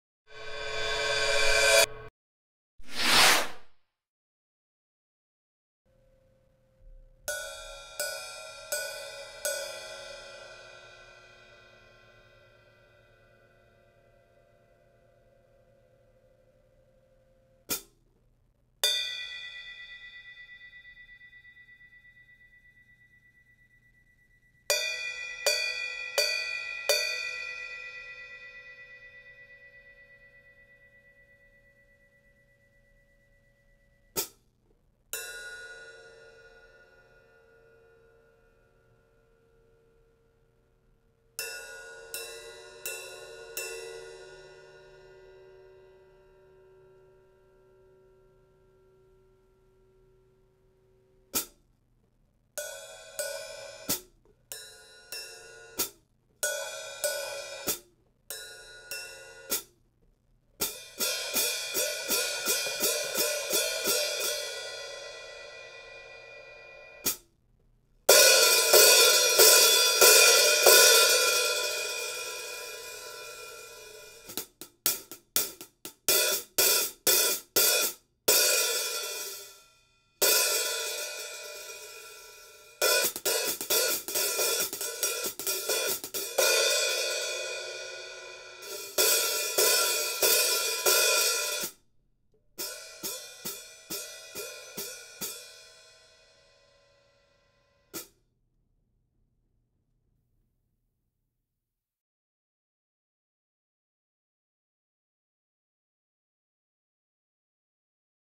Zildjian Sound Lab 14 A Custom Mastersound HiHats
Featuring the same crisp, rich and colorful sounds associated with the A Custom range, but with a bolder and full-bodied overall sound.

Hammered grooves in the bottom cymbal rapidly vent trapped air for a fast and clear "chick" sound.

Weight: Medium Thin Top / Medium Bottom
Finish: Brilliant Finish
Bell Size: Small
Profile: Medium
Pitch: Mid
Sound: Solid Chick
Volume: General
Balance: Cut
Sustain: Medium